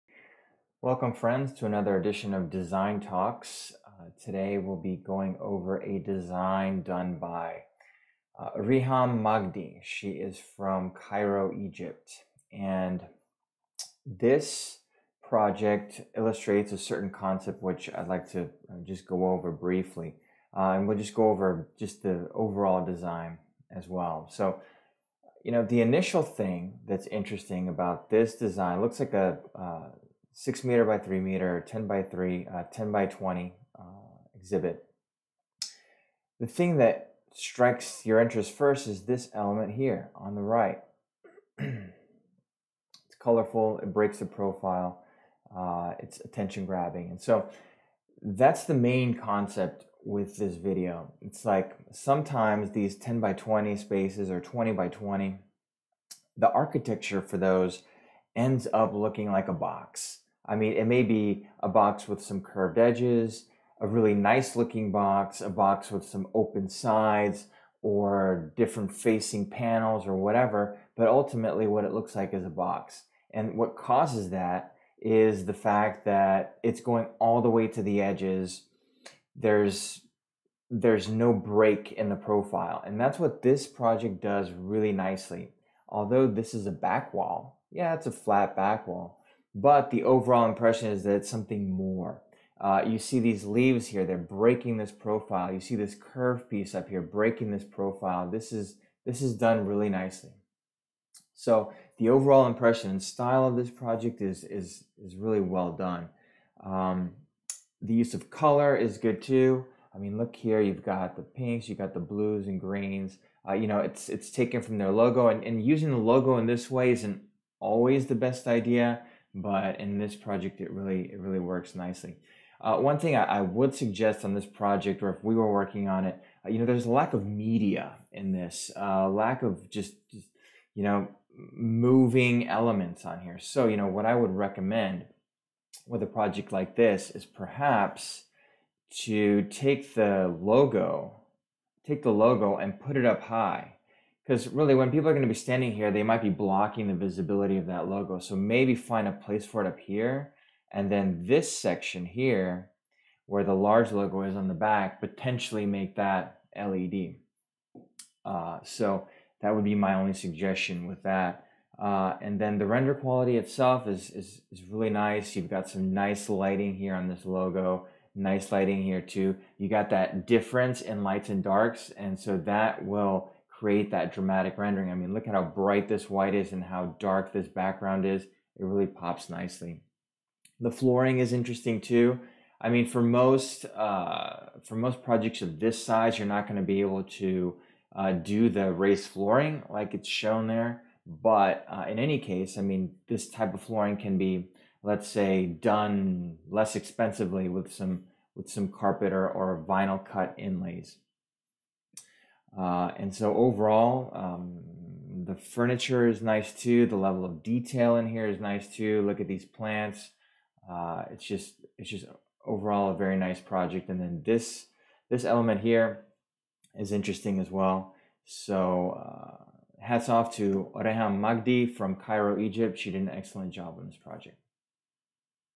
Design Talks: Breaking the profile.
Design Talks discussion of design by Reham Magdy, with a focus on actionable tips you can use in your own works.